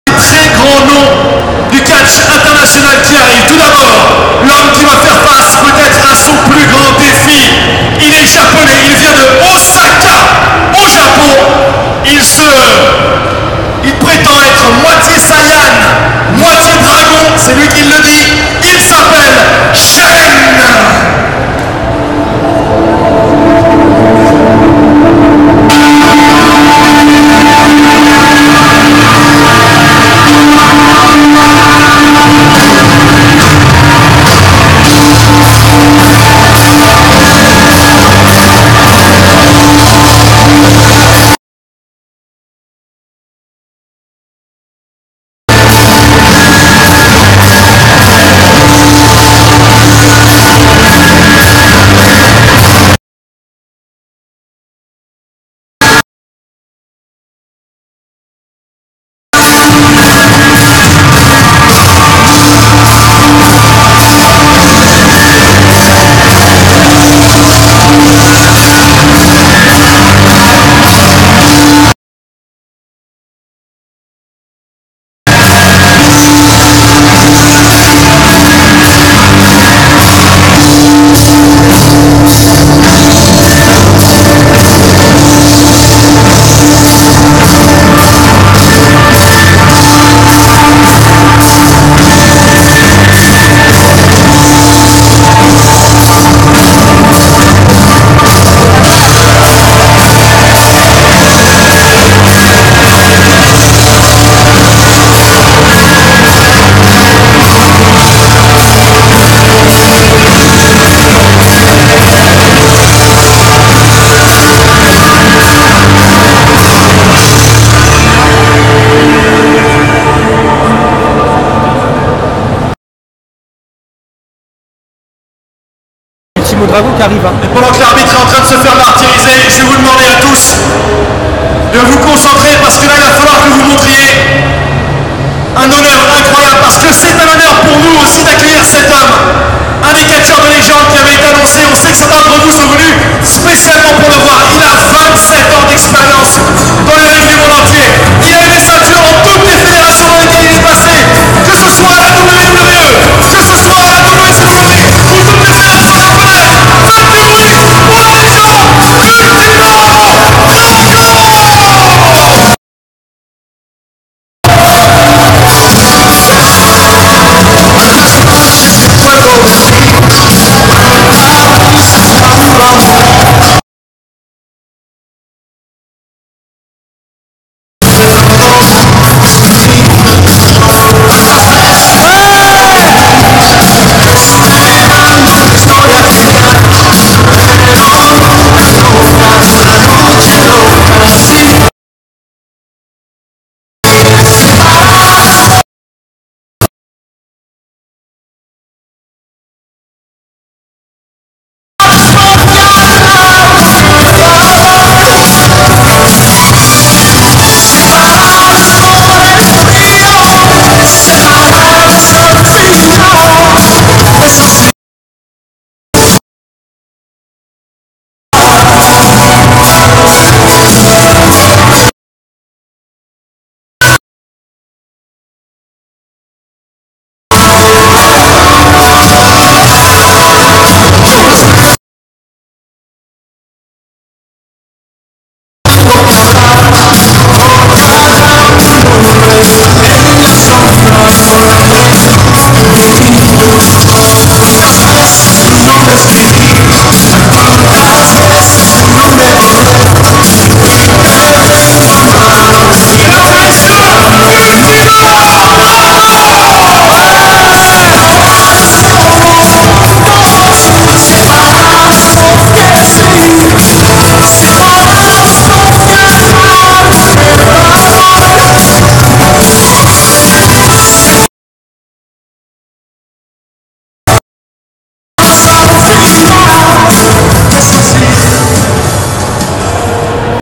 ULTIMO DRAGON's entrance at JAPAN EXPO: 07/03/2014
Wrestling  Legend Ultimo Dragon and his opponent  SHEN 's entrance  for their match at  Japan Expo on 07/03/2014